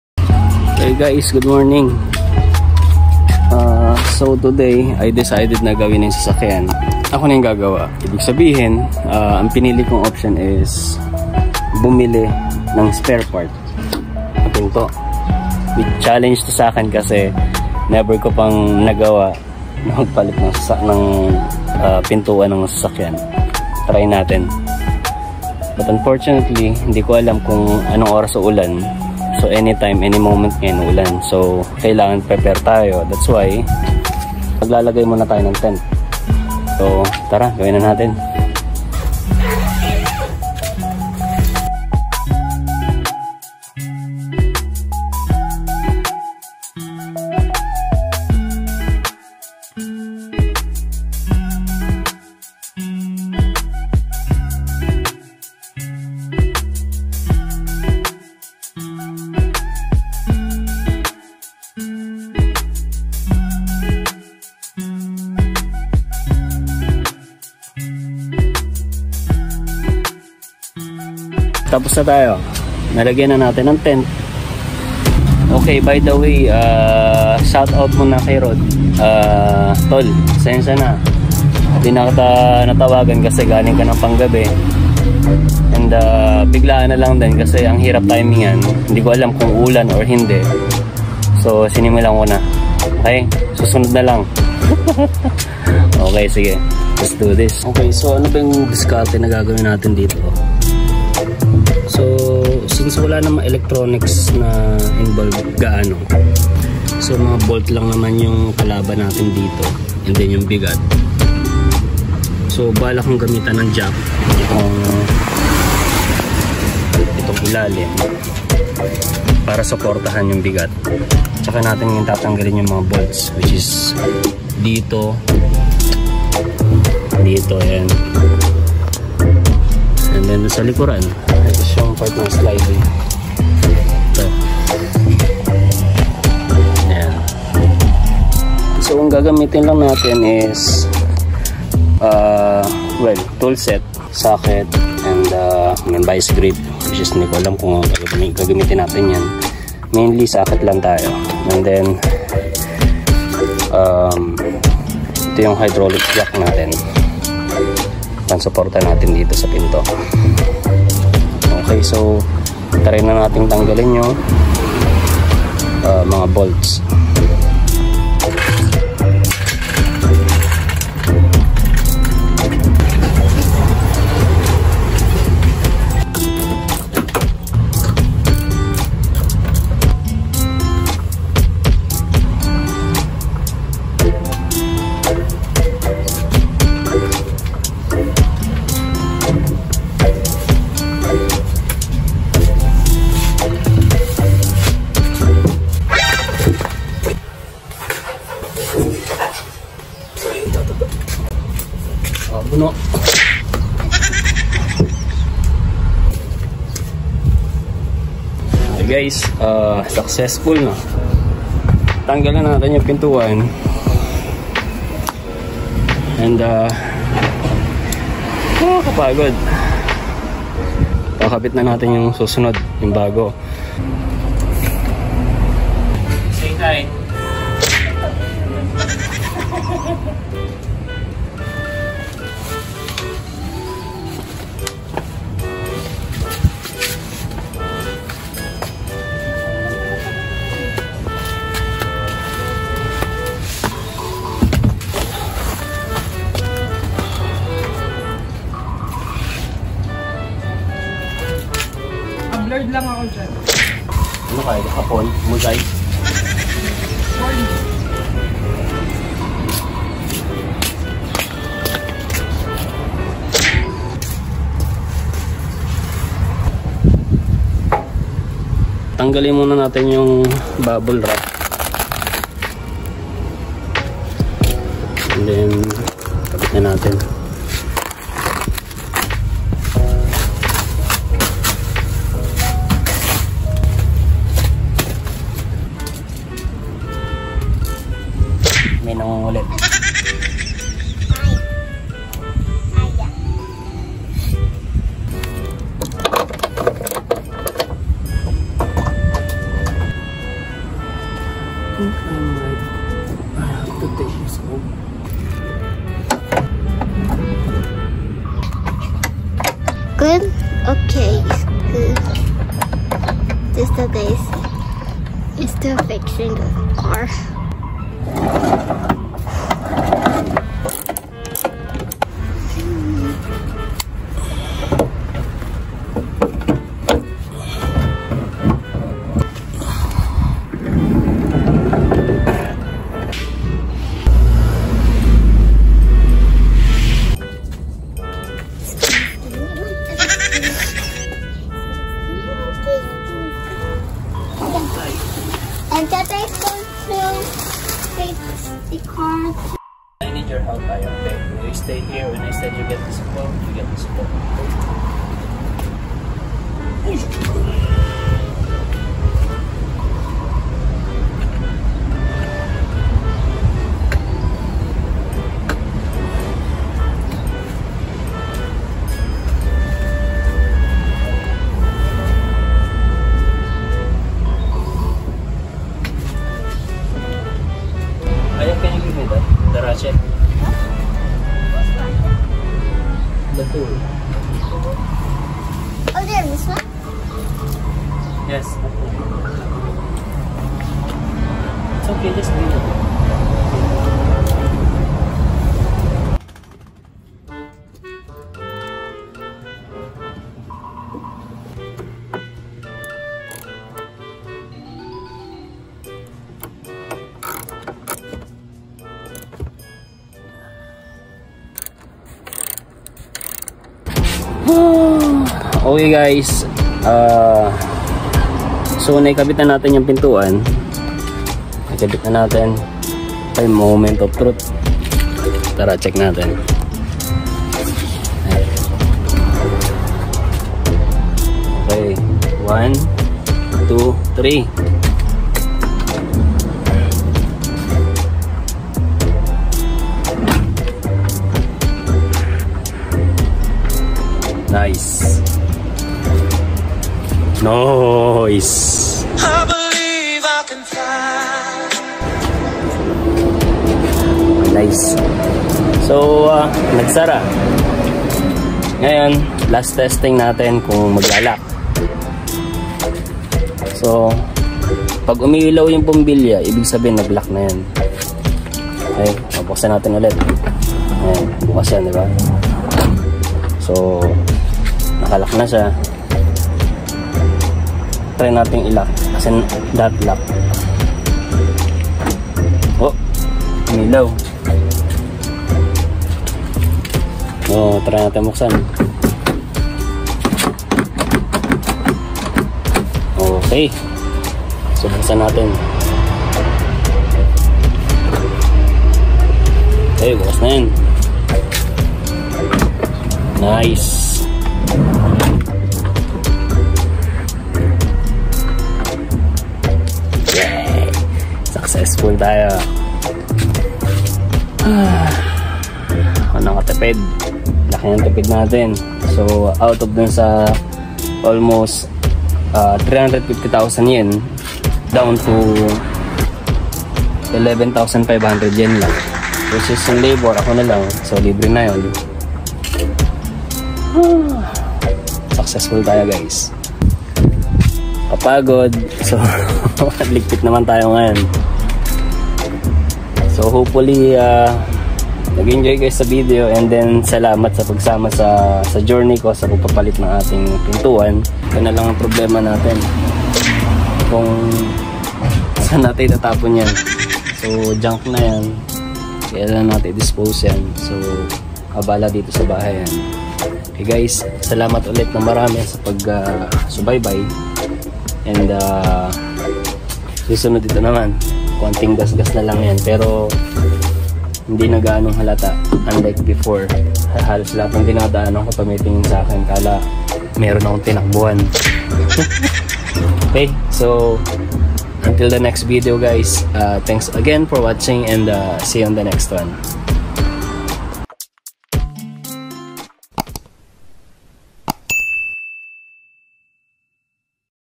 Repair ng Sasakyan | Buhay sa Japan
I finally found a way to fix my Car's door and It's a lot more cheaper than taking it to the shop and give them all the money for unreasonable price. It took me awhile before I finally decided to do things on my own.